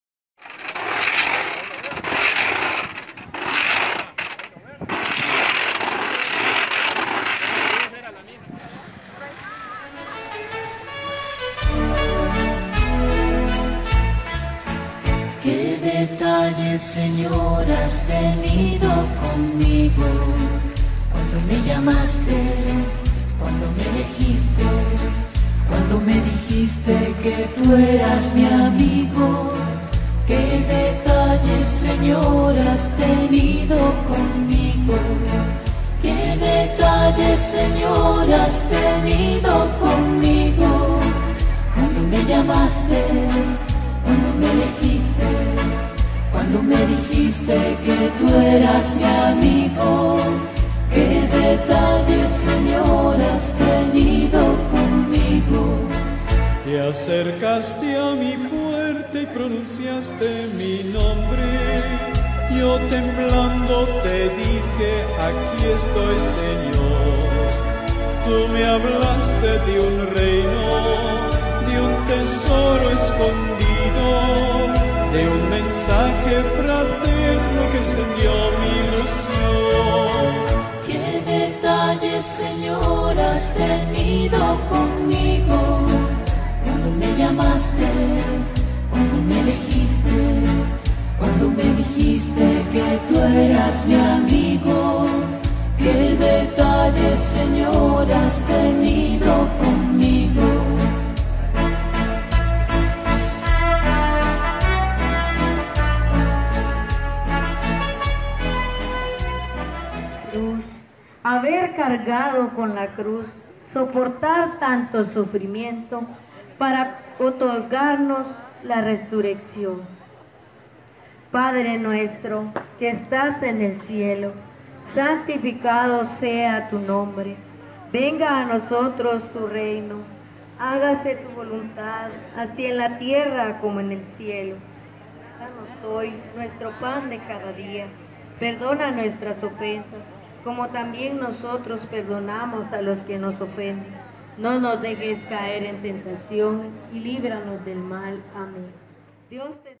Procesión viernes Santo en Esquipulas 2018
Santo Vía Crucis recorrió calles y avenidas de nuestra ciudad Esquipulas en este viernes Santo, día muy especial para la feligresía católica de Guatemala. Cientos de peregrinos acompañaron el cortejo procesional con la imagen de Jesús y la Virgen Maria Madre de nuestro Señor.